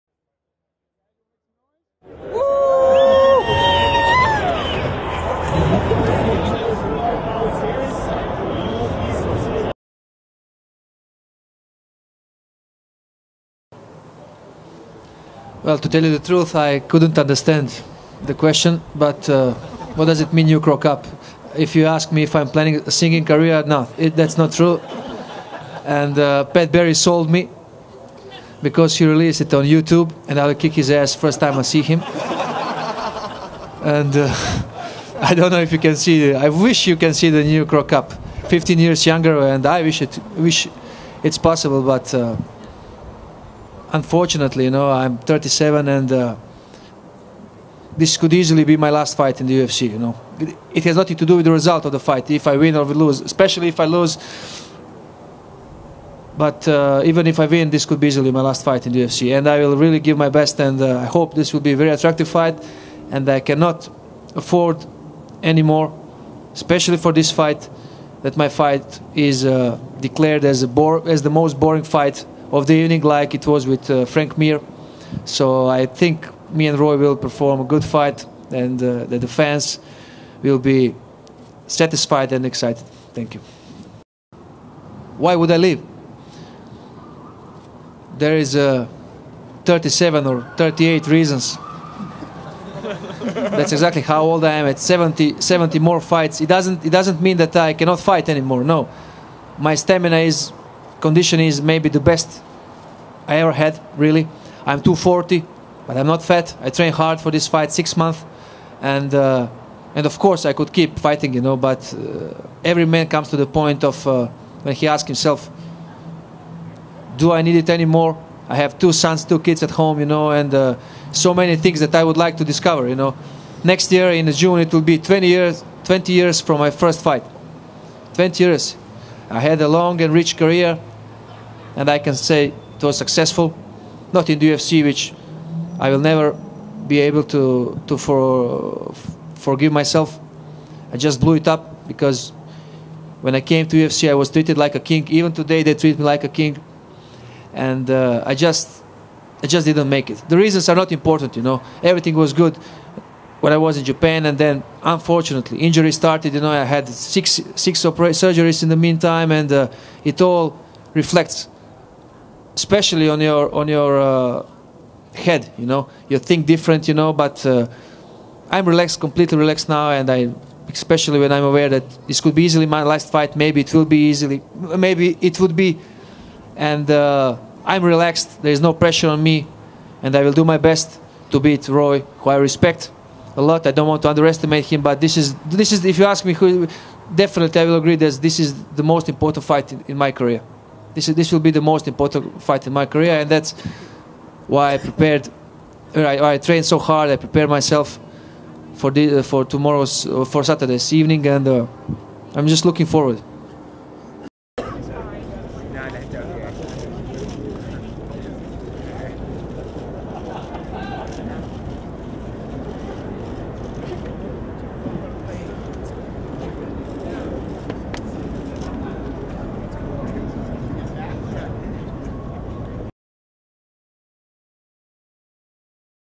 UFC 137's Mirko Cro Cop Wants To Kick Pat Barry's Ass For Releasing Singing Video
MMA H.E.A.T. brings you this excerpt from the UFC 137 pre-fight press conference, where Mirko Cro Cop answers a question about the video of him singing with Pat Barry. Apparently, that clip was never meant for public consumption, and Mirko plans to make Pat Barry pay for it. He also talks about possibly retiring after his fight with Roy Nelson and how he feels going into it.

for more interviews and archived episodes of our 1/2 hour TV show.